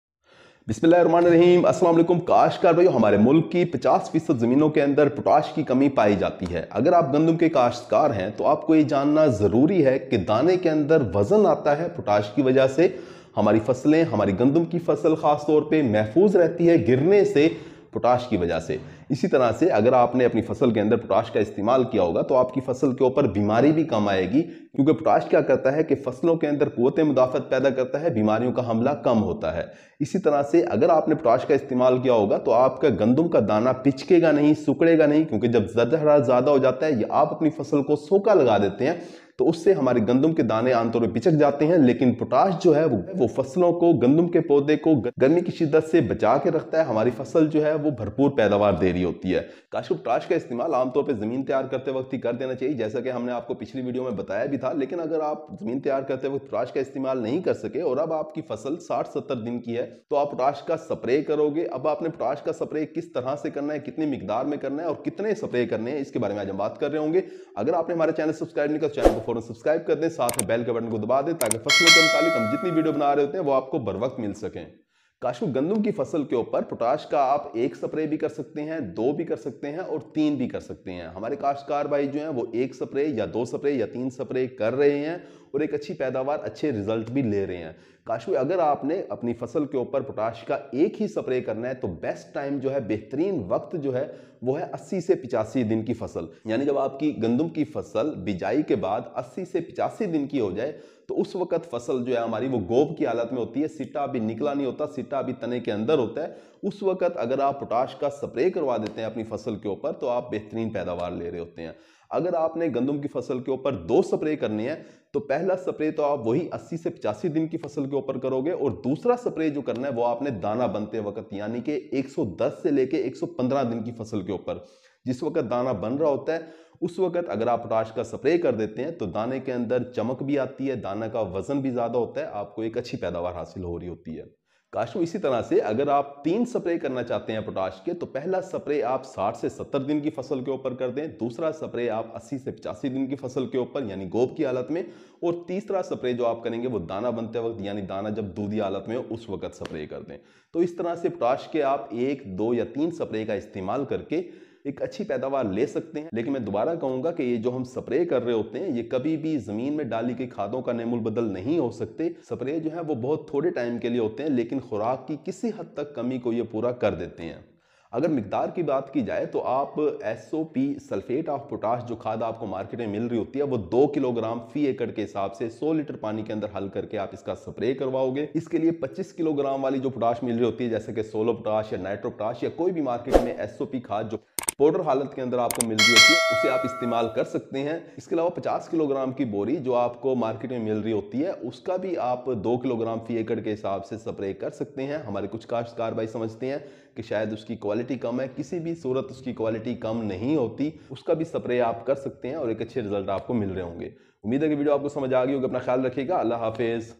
Potash spray benefits in wheat field |Potassium foliar spray application time and dose on wheat crop
Potash spray benefits in wheat field |Potassium foliar spray application time and dose on wheat crop |Abid Ali Agrarian

In this video, I will shortly explain the benefits of potassium nutrients in a wheat crop such as;
1) Potash fertilizer increases grains' weight and size.
2) Potash improves the strength of wheat stem so wheat fields can be prevented from lodging by application of potassium fertilizer.
3) Potash SOP or MOP fertilizer increases the defensive system of crop plants against diseases.
The best method of potash use is soil application at the time of land preparation but farmers can also apply it through leaves such as a foliar spray of potash fertilizer.
Potash powder from sources Solupotash, SOP 25 kg bag, or SOP 50 kg bag can be used for Foliar spray.